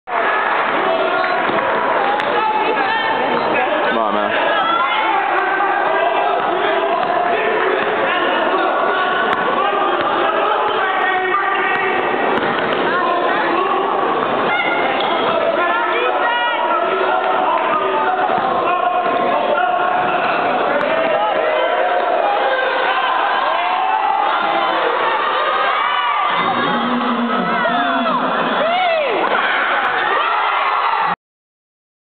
Ricky Cadell's 2-point jumper puts the Terriers ahead of Wagner
Jan 22 - SFC vs Wagner College - Ricky Cadell puts the Terriers ahead by 3 with under a minute remaining as the Terriers hold on and win 65-62.